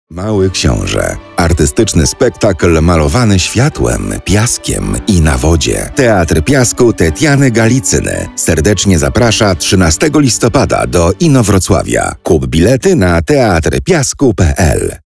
Mały Książę : Rodzinny Artystyczny Spektakl Teatru Piasku Tetiany Galitsyny w Inowrocławiu
Bilety na rodzinny spektakl Mały Książę Teatru Piasku Tetiany Galitsyny. Wybierz najlepsze miejsca z planu sali.

13 LISTOPADA 2019r. Inowrocław, Kujawskie Centrum Kultury, godz. 18:30

Widziałeś to w „Mam Talent”? Teraz możesz zobaczyć to na żywo!
Teatr Piasku Tetiany Galitsyny, zwyciężczyni programu „Mam Talent”, przedstawia artystyczne widowisko dla całej rodziny: „Mały Książę” niezwykłe show malowane piaskiem, światłem i na wodzie połączone z grą aktorów.
Przyjdź i zobacz artystyczną interpretację ponadczasowej opowieści!

GALITSYNA Art Group